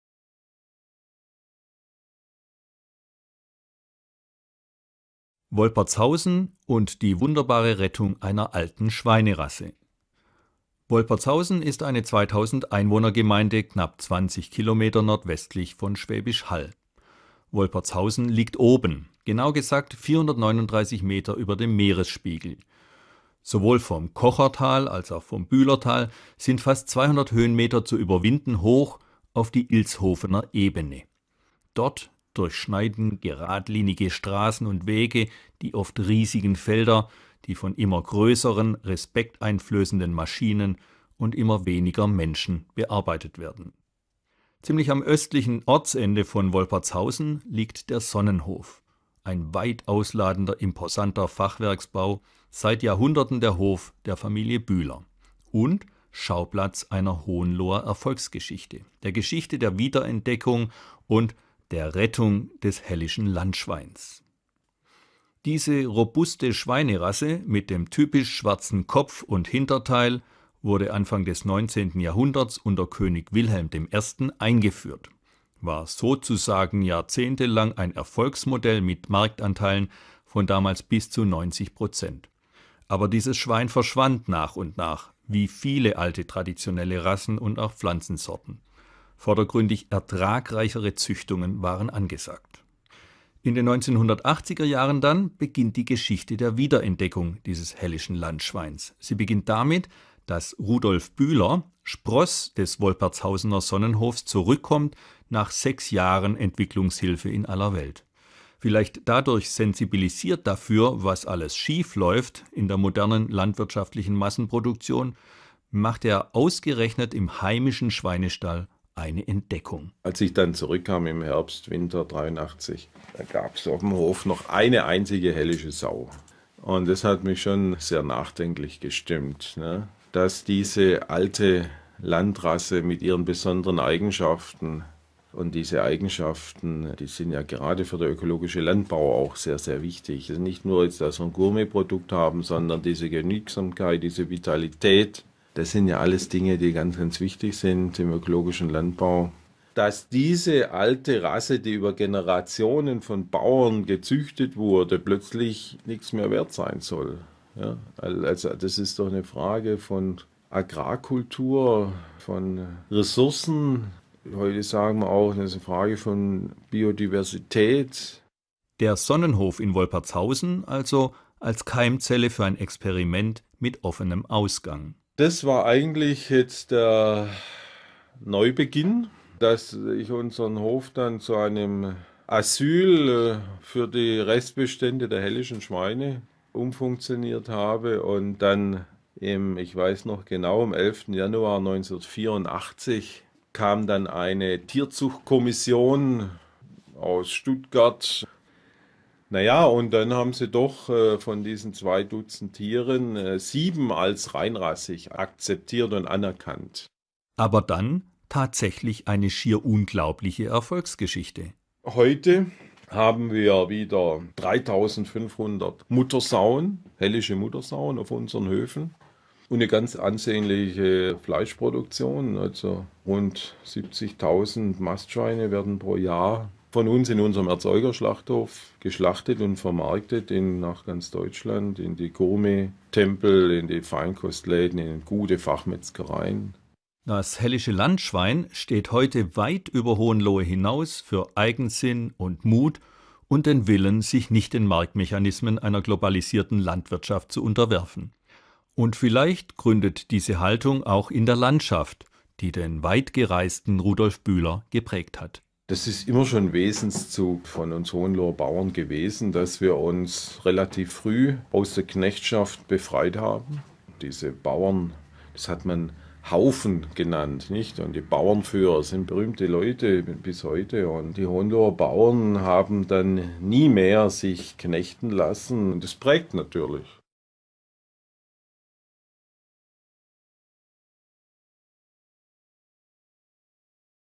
Hohenlohe und Tauberfranken - Folge 2: Wolpertshausen und die wunderbare ...
Hohenlohe und Tauberfranken - Folge 2: Wolpertshausen und die wunderbare Rettung einer alten Schweinerasse

Multimedialer Reisebegleiter

Eine Initiative zur Umwelt- und Nachhaltigkeitsbildung
der Akademie für Natur- und Umweltschutz Baden-Württemberg
realisiert von Thomas Hagenauer